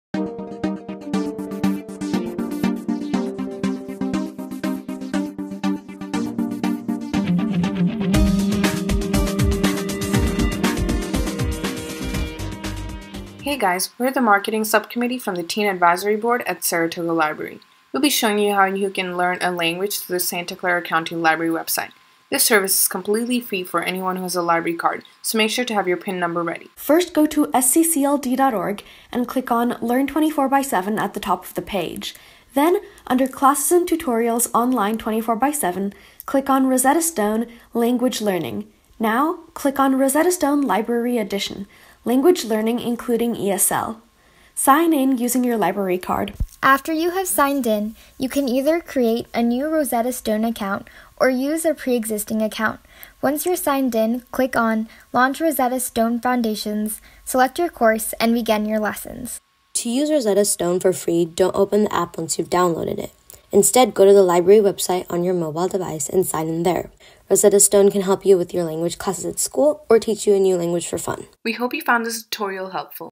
Teens STREAM+: Learn a Language with Rosetta Stone
Learn how to access Rosetta Stone through the Santa Clara County Library District's website.